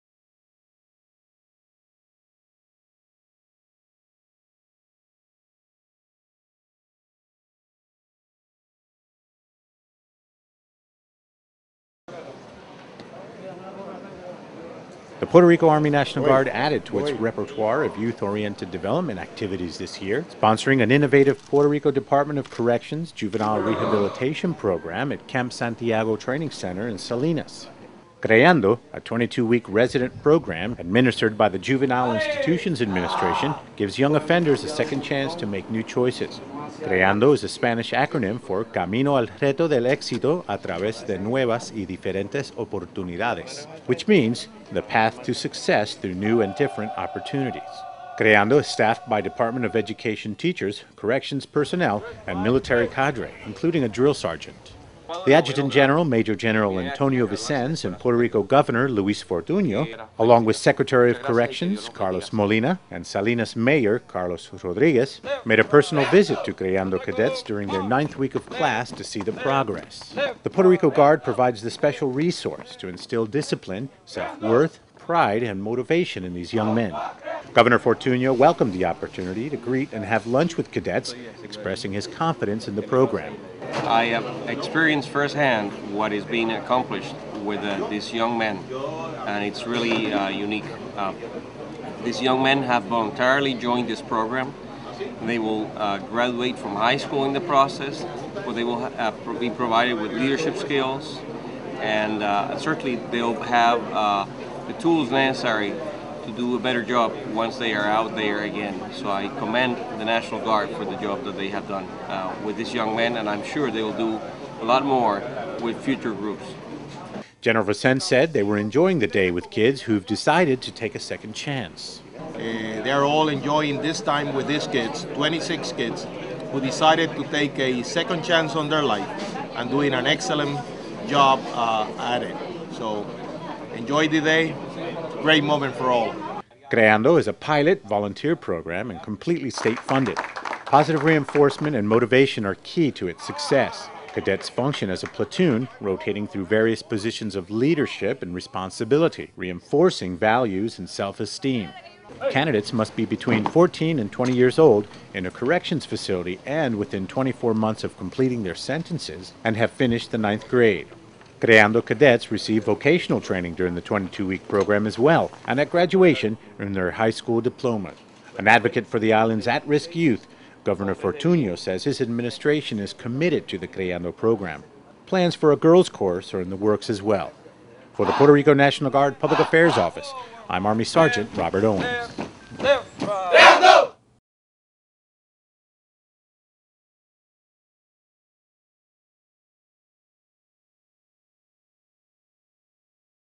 Governor visits CREANDO
Governor of Puerto Rico Hon. Luis Fortuño visits the CREANDO program.